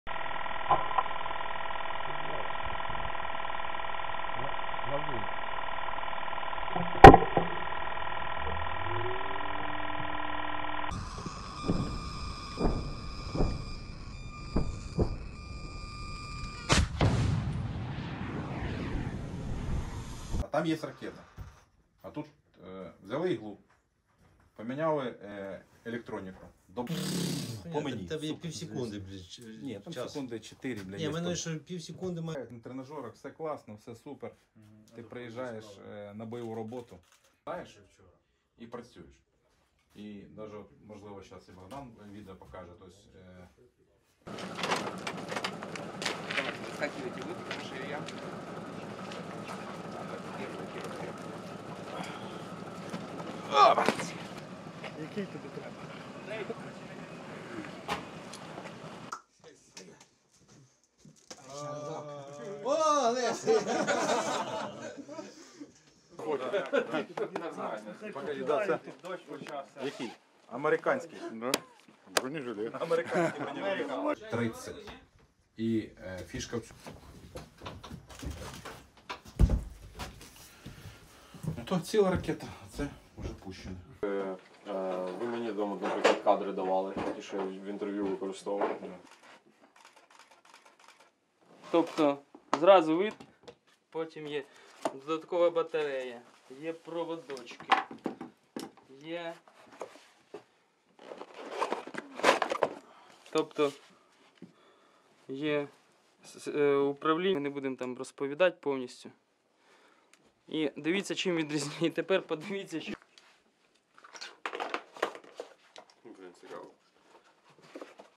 Ukrainian soldiers fire anti-aircraft weapons to shoot down Russian drones
Ukrainian soldiers said on Saturday (December 17) that they were ready to fight Iranian-made drones that Russia uses to target Ukrainian cities and infrastructure.

Despite having no previous combat contact with “Mopeds”, as Ukrainians call Shahed-type drones, Denys, a commander of a Ukrainian air defence unit, was sure that his officers would be capable of taking them down.

The delivery of Western anti-aircraft weapons to Ukraine's armed forces makes it harder for Russian pilots to fly closer to targets, Denys said.